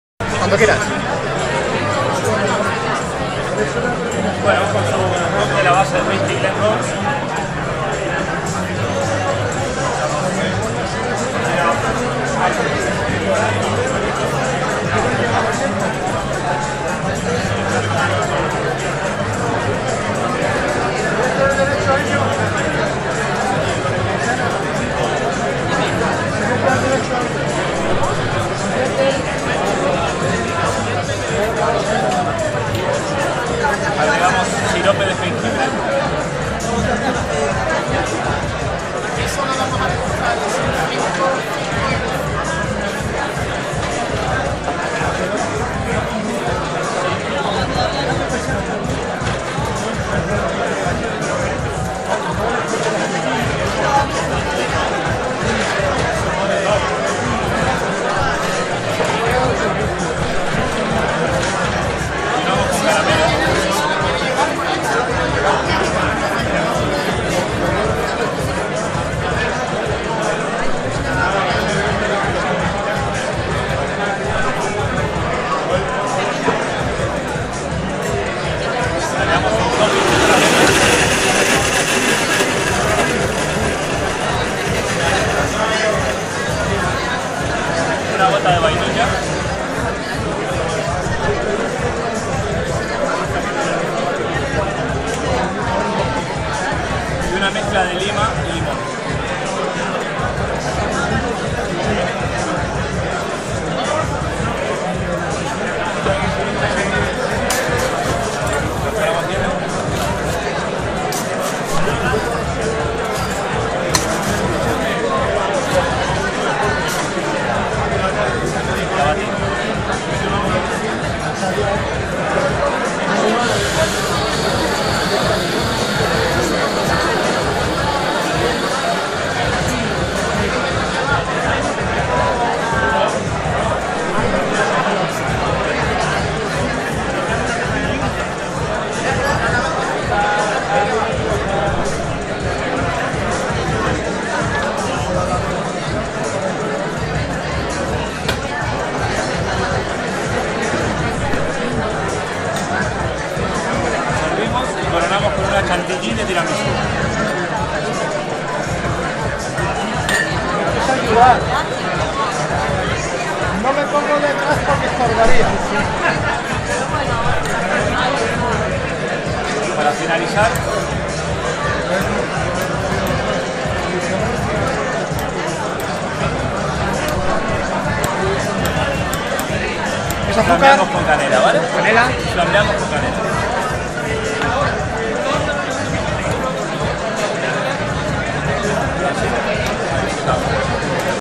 Original Rothes.mp4
Cóctel Original Rothers. Preparación durante el VI Whisky Festival en Madrid.